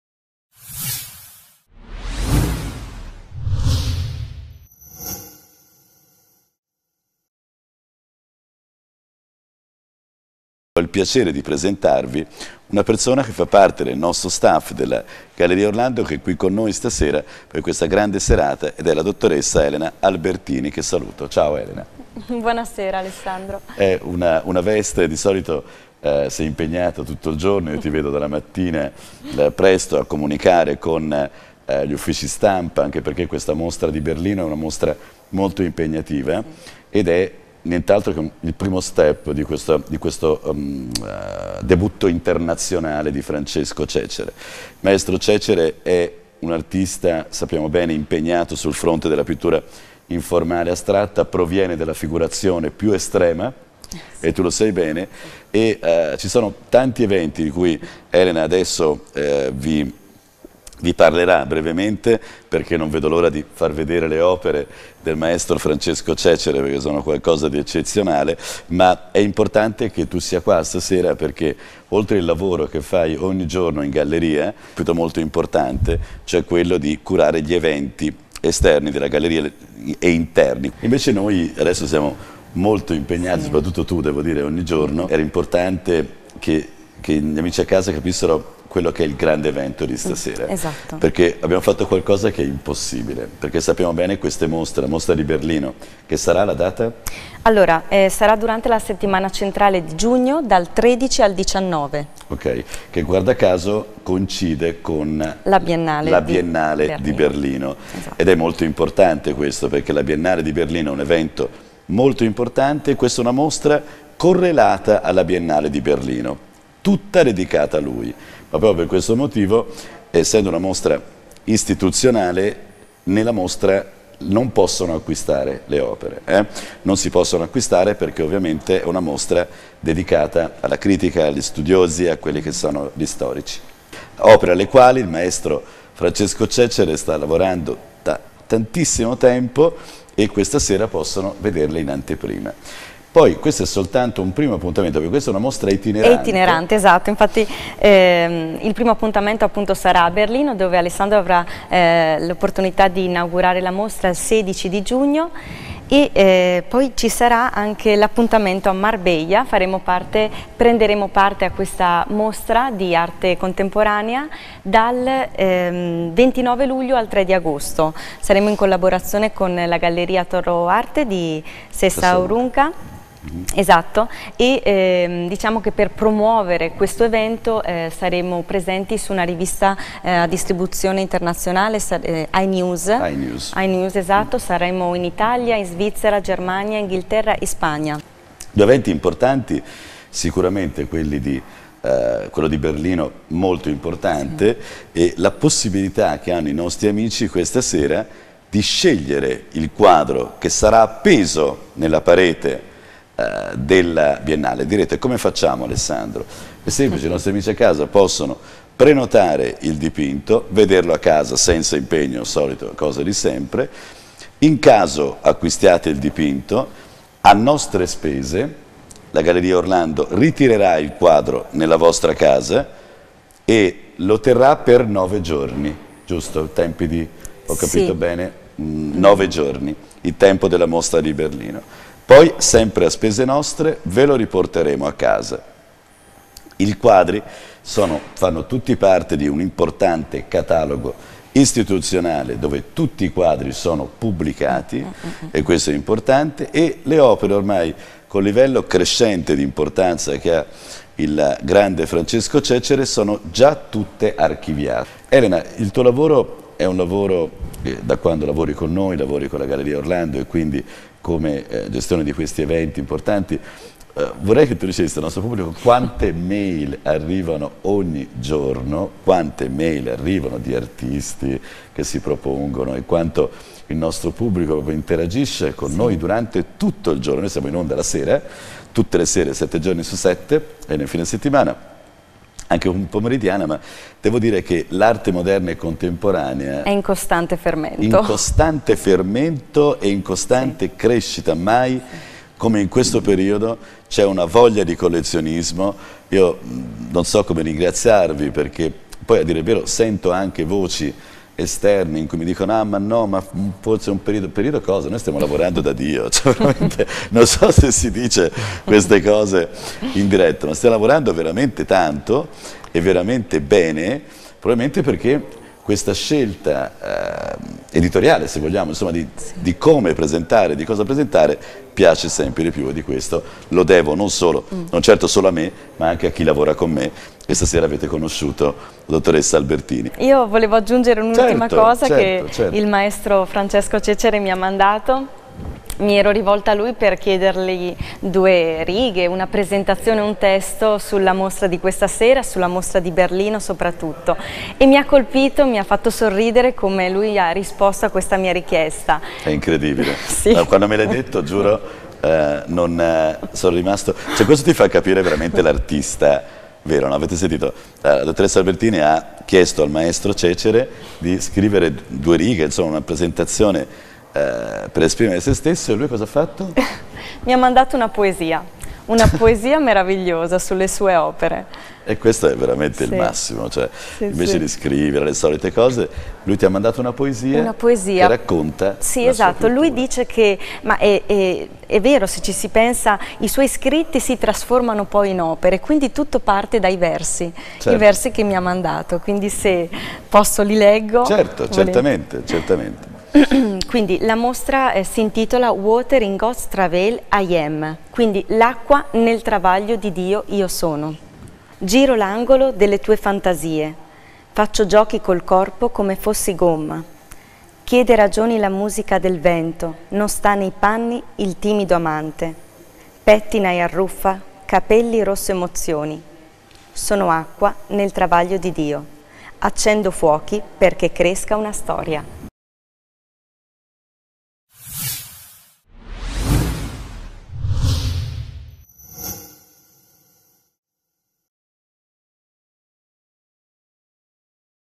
Dr.ssa Elena Albertini
Una breve intervista con la Dr.ssa Elena Albertini dove spiega la mostra di CECERE